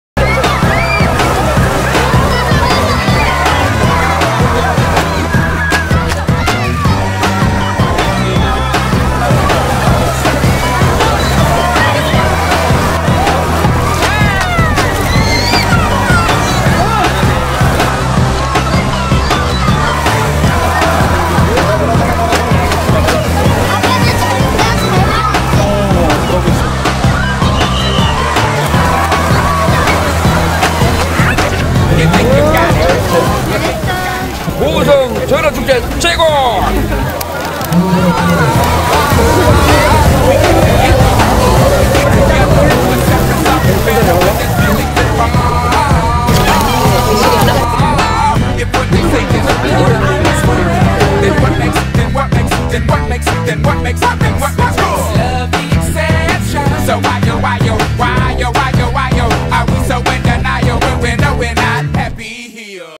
율포 활어잡기 페스티벌
율포솔밭해변에서 활어잡기 페스티벌이 열린다.
2019.9.7.까지 매주 토요일 오후 2시 보성 율포에서